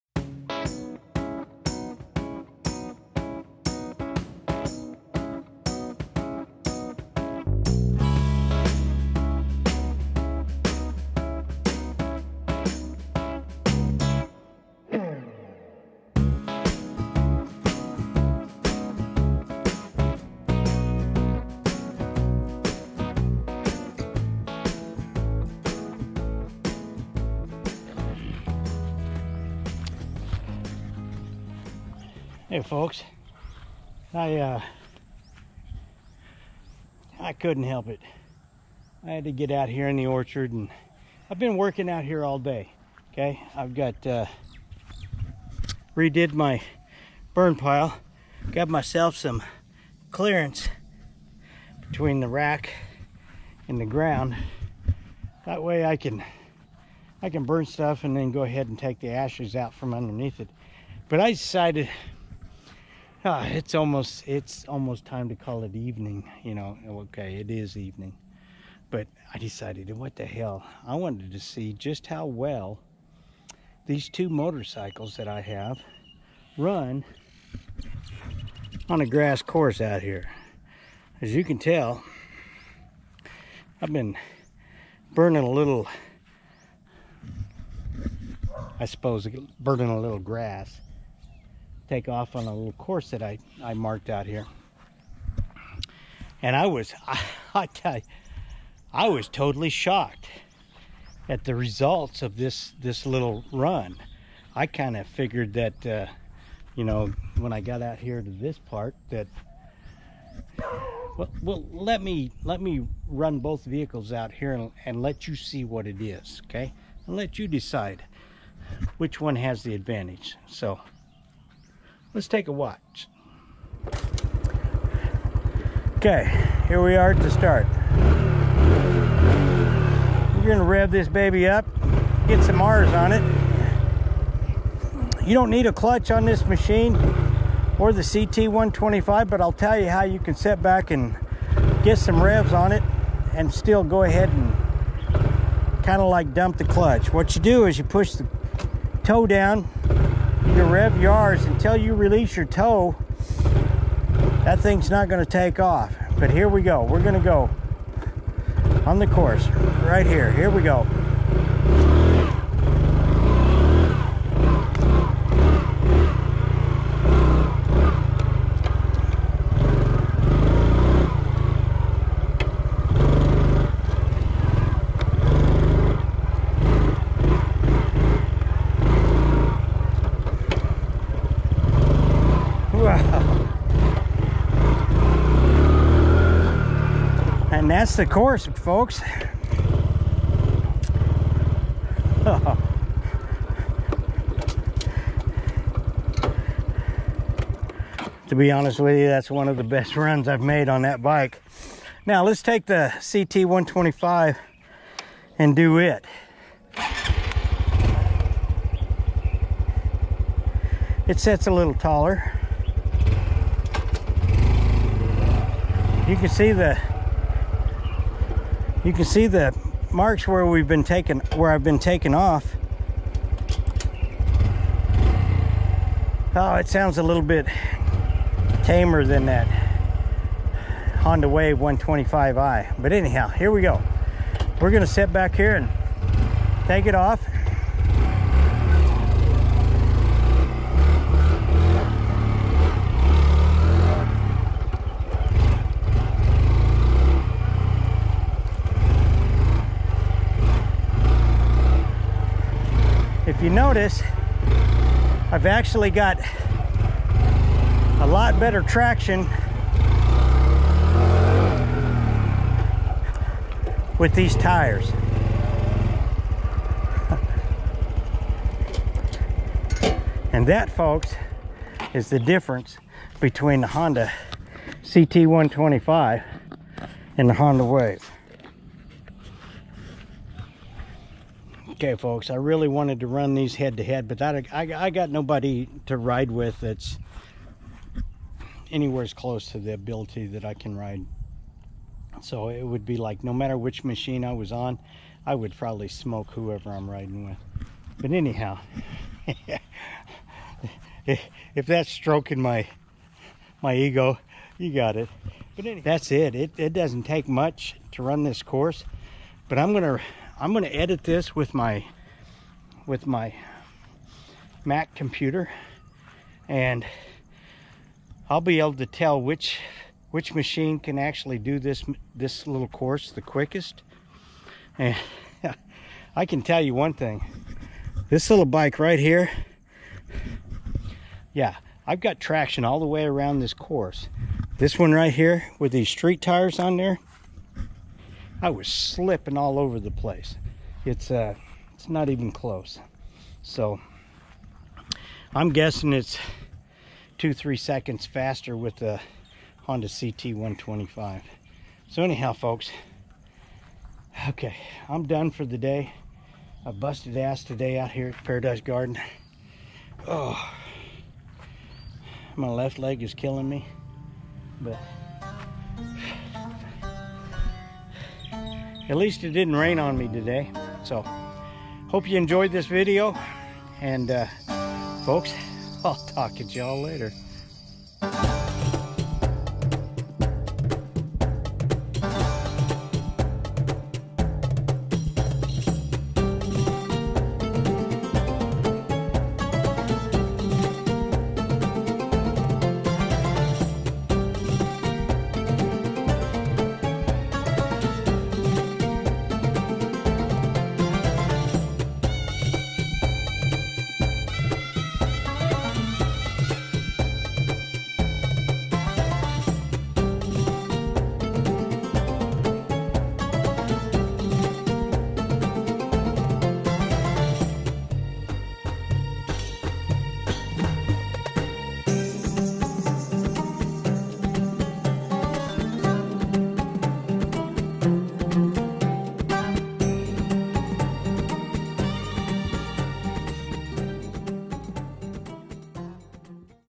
Honda CT125 & Honda Wave 125i Head to Head Scramble Track
This video is about Honda CT125 & Honda Wave 125i Head to Head Scramble Track A viewer wanted a head to head with my two bikes, so I played out a little grass track in the orchard and twisted the grips. Here is the results. The result times are at the end.